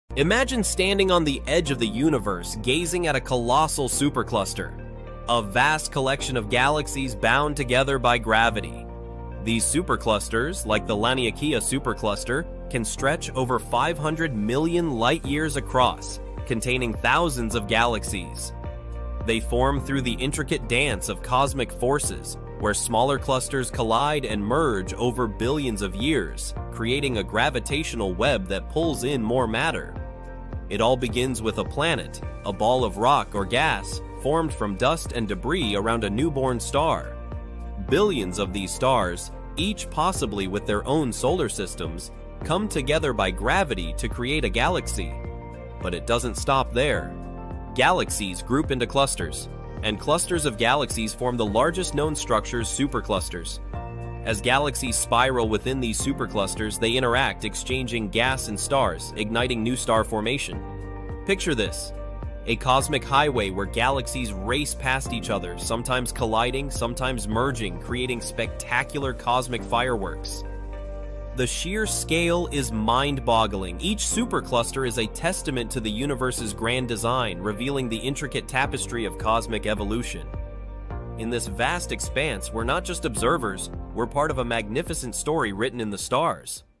What's Hiding in the SUPERCLUSTER?
Join me as I dive deep into the mysteries of the supercluster and see if I can uncover what’s really hiding out there!
Explore the mysteries of the universe with us as we delve into the fascinating world of superclusters! A supercluster is a large region of space that contains many galaxy clusters, but what lies hidden beneath the surface? From dark matter to unseen galaxies, we'll uncover the secrets that are hiding in plain sight. Join us on this epic journey through the cosmos and discover the wonders that await us. What secrets will we uncover? Watch to find out!